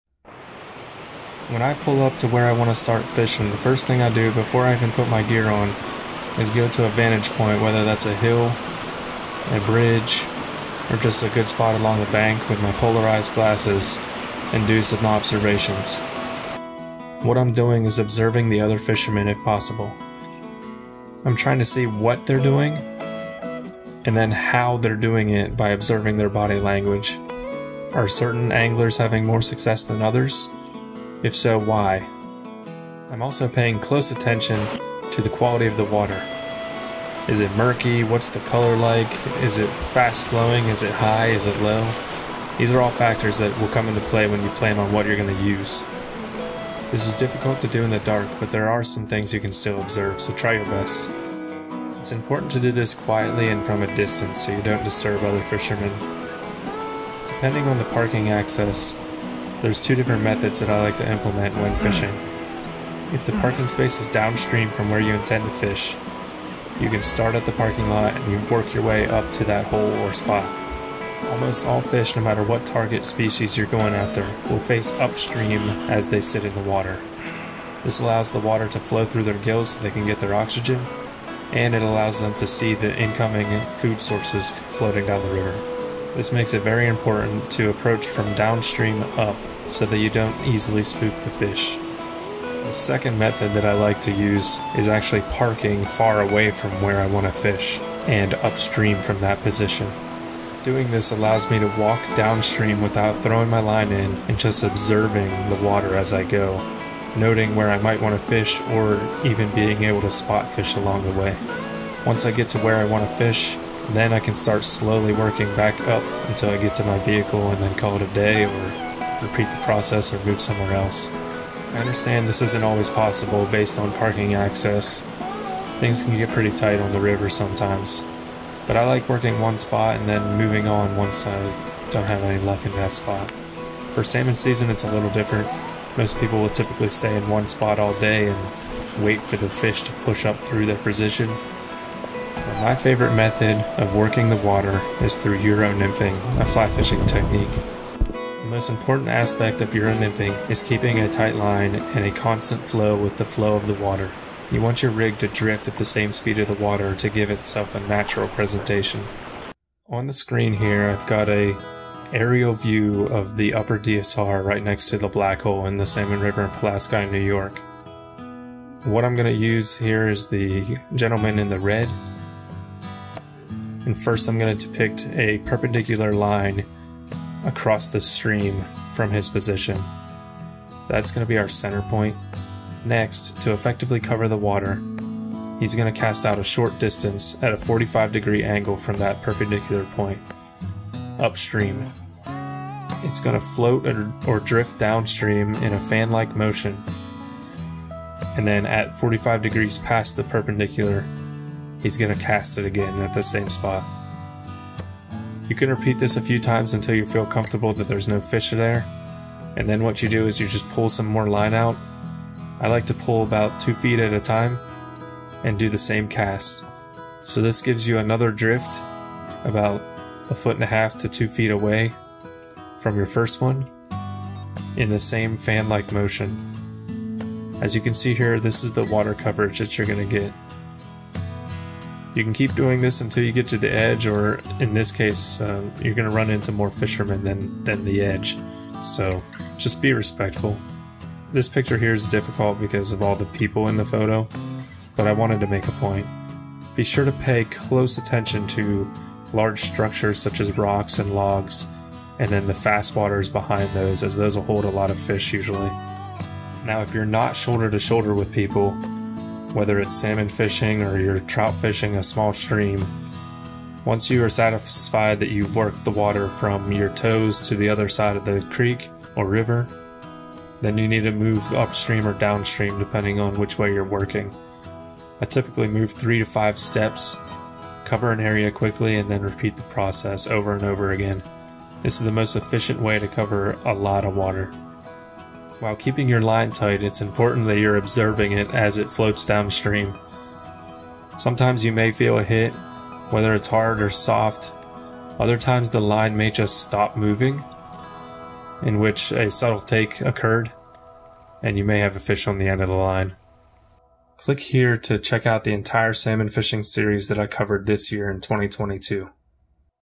Fishing Tactics that Will Make You Better
Now that we know what gear to use and have some hooks in our fly boxes, it's time to talk about how to approach the whole experience. This includes observing people and the water they are fishing.

I'll cover what I do each trip and briefly go into why I do it. There are thousands of ways to go fishing, so find what works best for you. I'll also go over some very basic Euro nymphing techniques.

In this series, I'll be covering many short topics to help improve your experience on the water this fall. Although this series is intended for Salmon, most of these techniques and practices may apply to other fishing types and seasons depending on the environment.

⏱️ Timestamps:
0:00 Pre-Observation
0:57 Options Based Off Parking
2:14 Peak Season Disclaimer
2:22 Euro Nymphing Basics
2:39 Visual Example/Breakdown
4:16 Real Life Demonstration